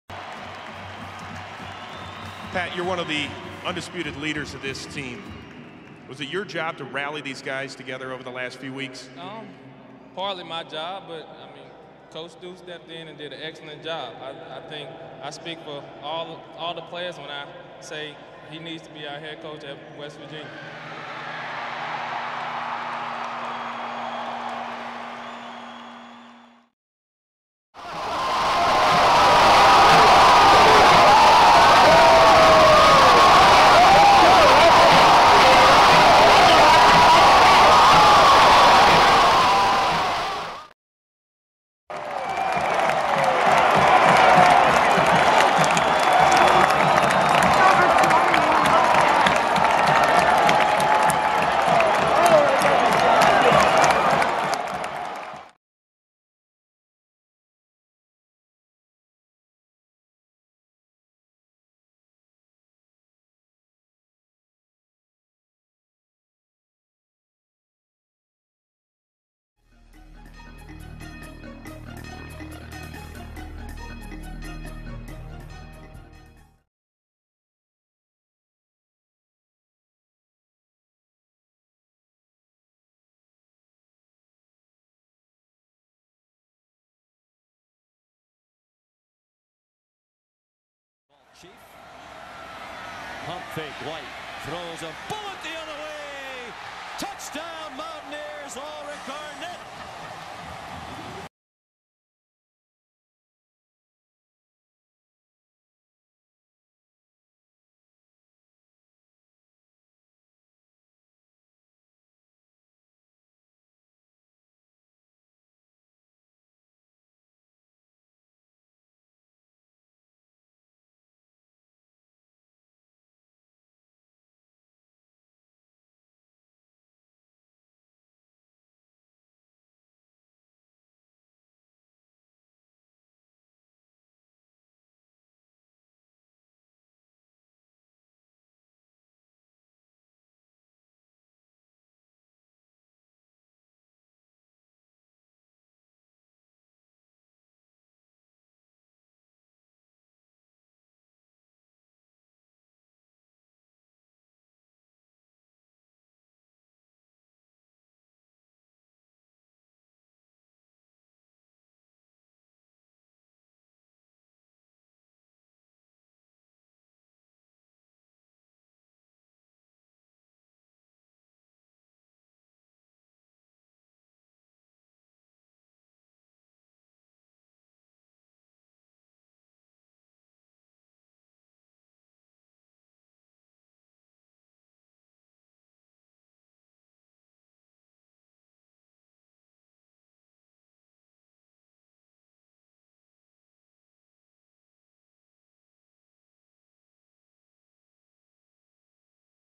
A Short History of WVU Coaches Since 2008
A brief history of the events in WVU coaching history since the departure of Rich Rodriguez.